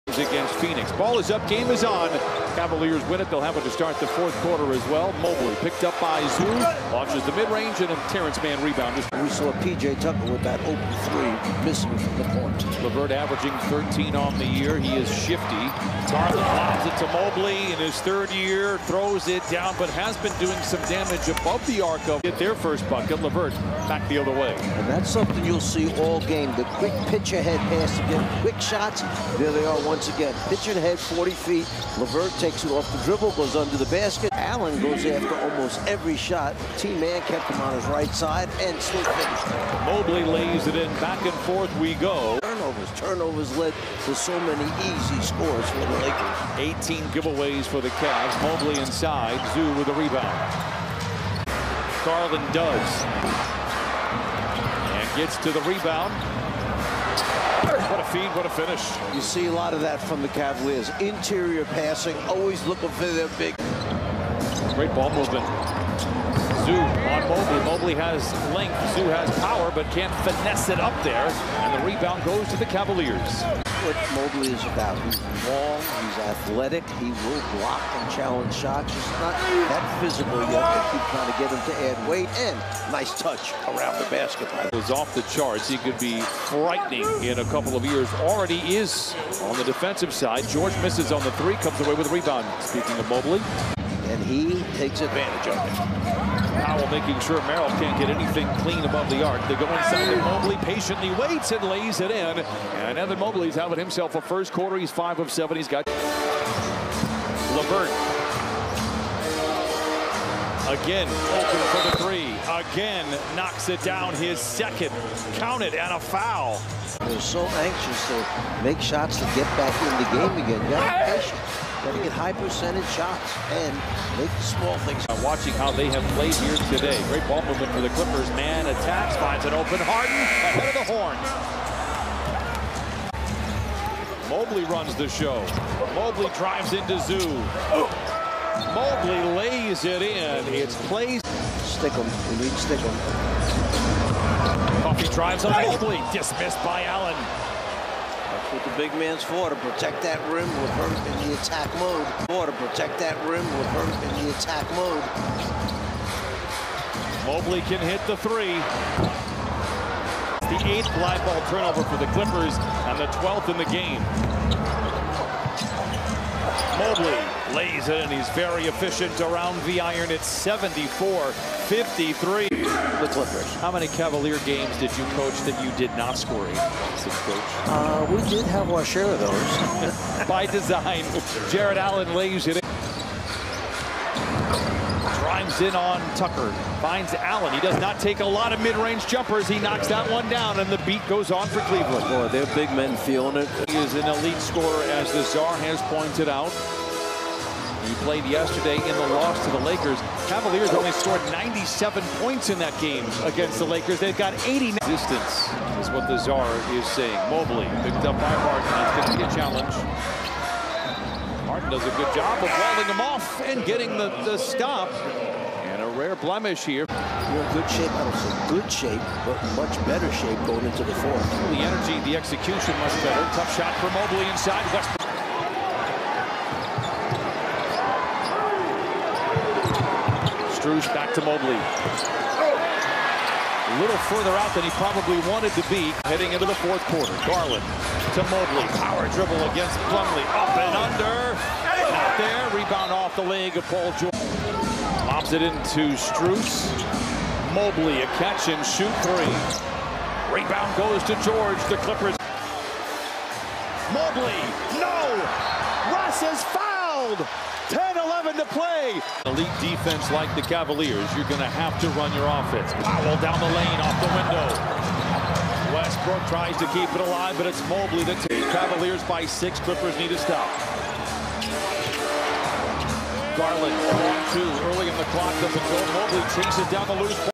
Evan Mobley player Full Highlights vs CLIPPERS NBA Regular season game 07-04-2024
Check out the latest highlights of Evan Mobley from the NBA preseason game between CAVALIERS vs CLIPPERS. Watch as Evan Mobley was preforming on the court with his field goal attempts, rebounds, turnovers, steals, assists, and blocks. This video includes all of Evan  gameplays possessions from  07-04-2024.